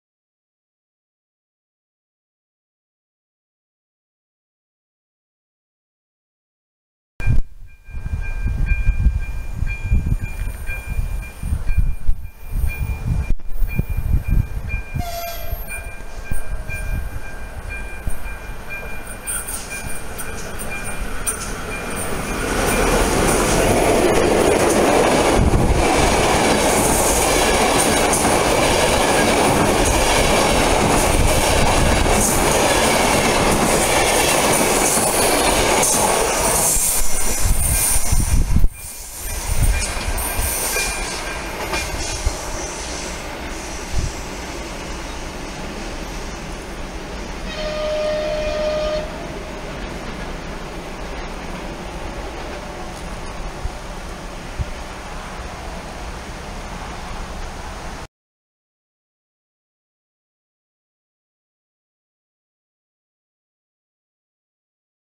90. RJ 1003 v stanici Oščadnica, 26.8.2020
Žltý vlak spoločnsti RegioJet natočený na zastávke Oščadnica. Rušňovodičovi ďakujem za pozdrav!

Vlaky:
1. RJ 1003 RegioJet (Praha hl. n. - Košice)